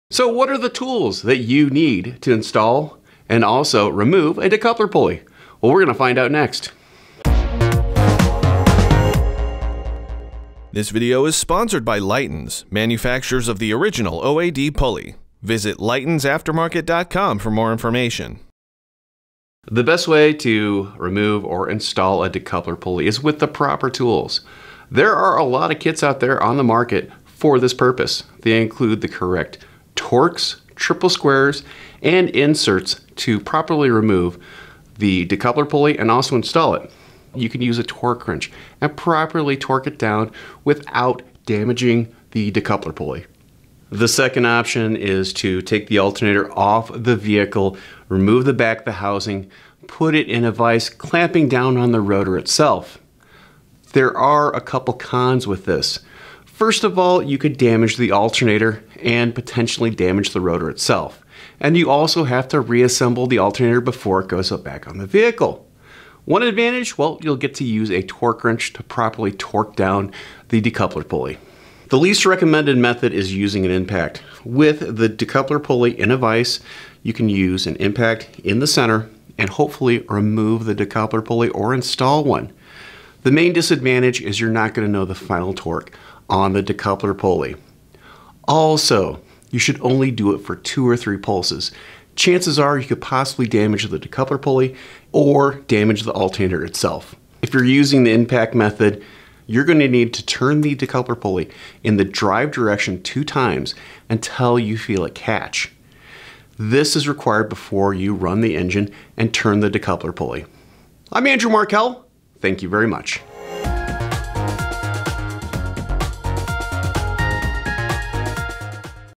Decoupler Pulley Installation & Removal Methods | Tech Minute
Andrew Markel discusses several installation and removal processes for decoupler pulleys, including which will produce the most reliable results.